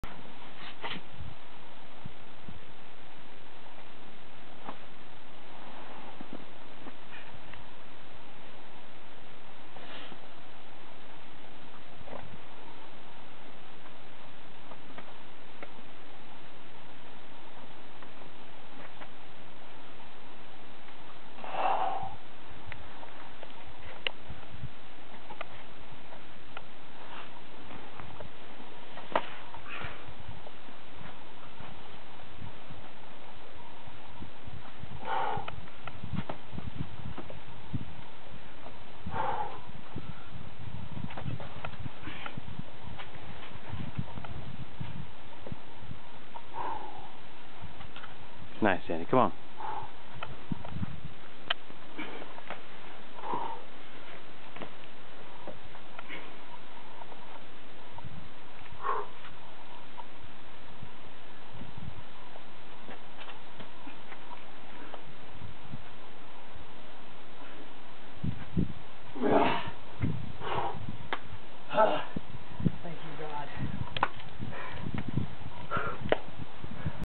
The Vagina, Rocktown, Georgia - The Send
Danny Spollen, future doctor extraordinaire, crushes this Rocktown Classic